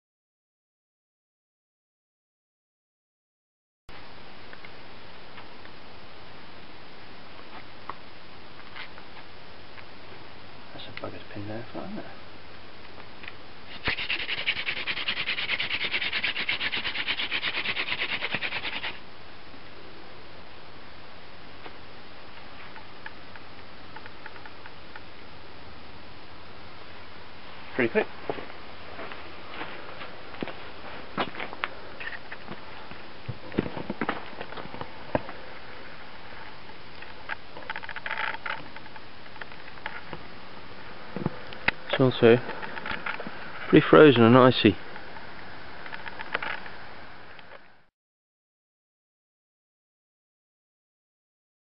5 second ember in the frost- fire by friction hand drill.
Frosty evening using elder on sotol with the pressure run technique (shallow floating and pushing down hard at speed to get lots of heat) got an ember in just under 5 seconds. Outside temp was minus 4.